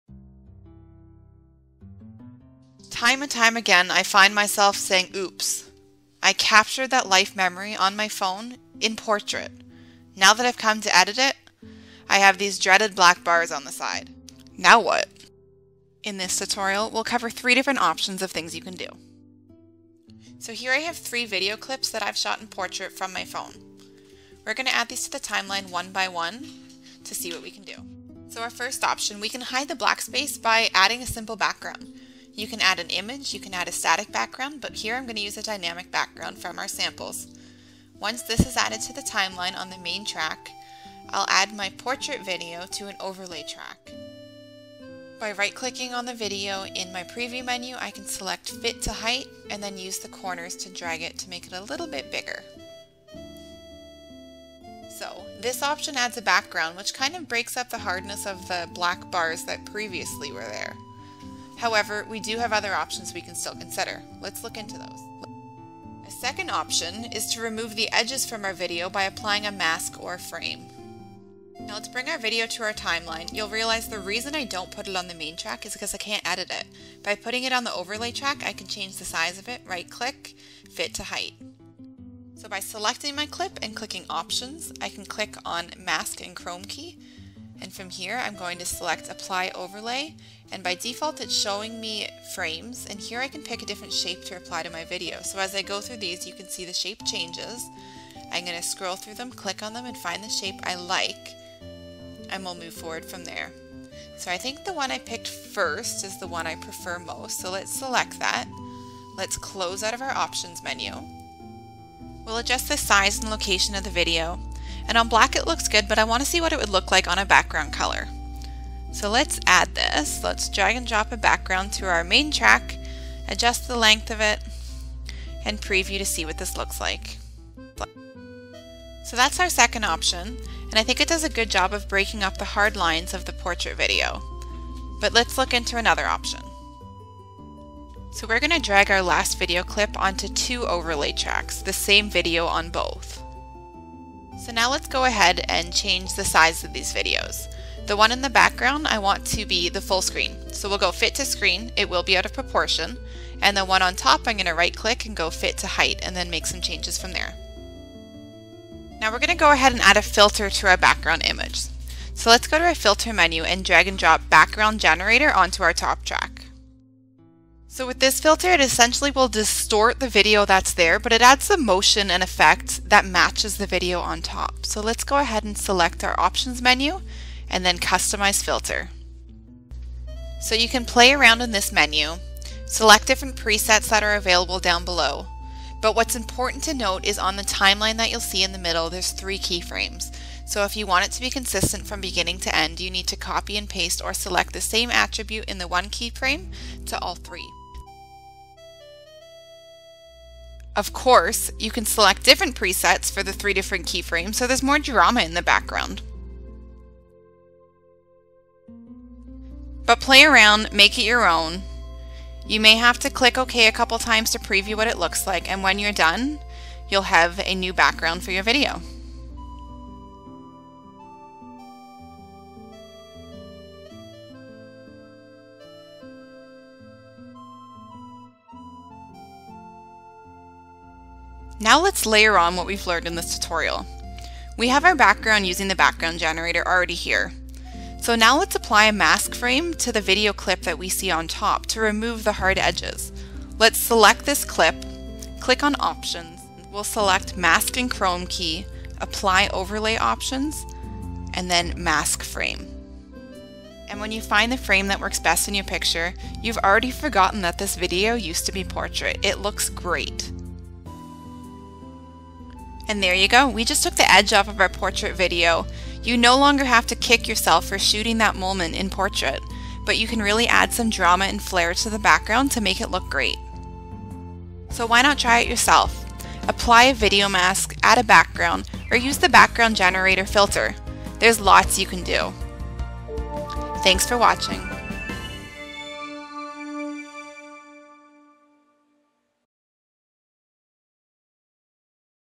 Working with a Portrait Video in VideoStudio Ultimate – applying masks and creating backgrounds
In the age of mobile devices, we often find that we captured that moment in portrait – and now have to work with those black bars on either side of our video.  Join us as we learn about Video Mask Overlays and how to create backgrounds with the NewBlue Background Generator.  For more information or to download a trail,